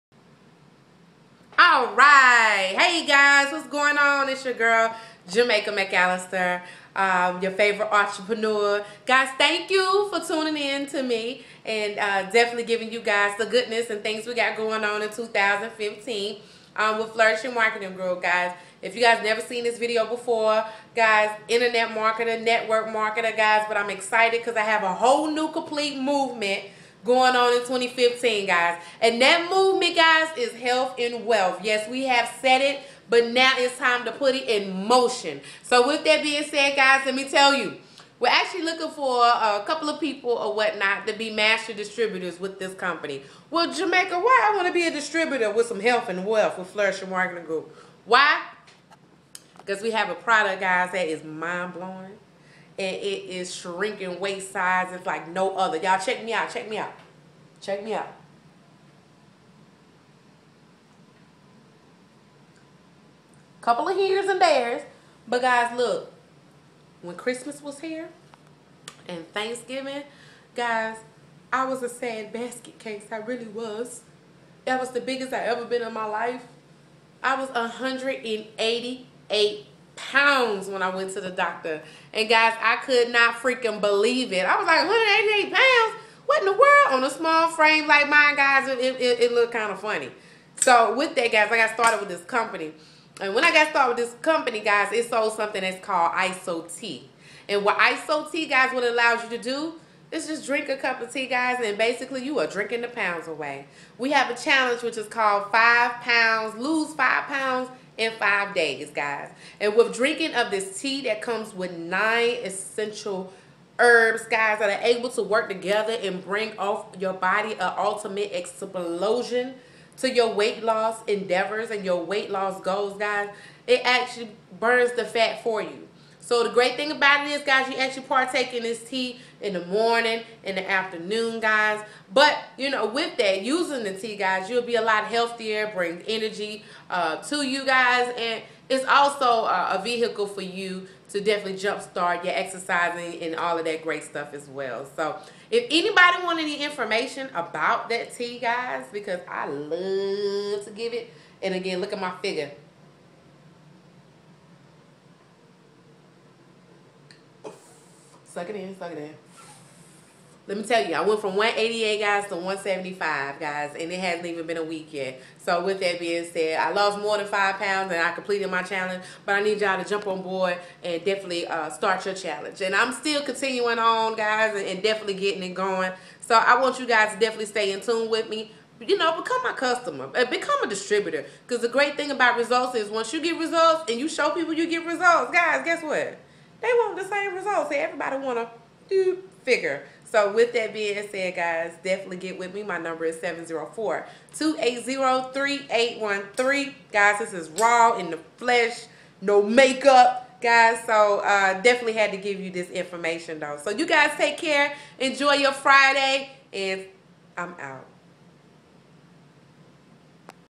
Jamaica McAllister Drops the Tea
Something New! Entrepreneur tells the followers about her new venture! This company is amazing and helps you curve your waistline and increase your bank account. I love ISOTEA get your product here today!!!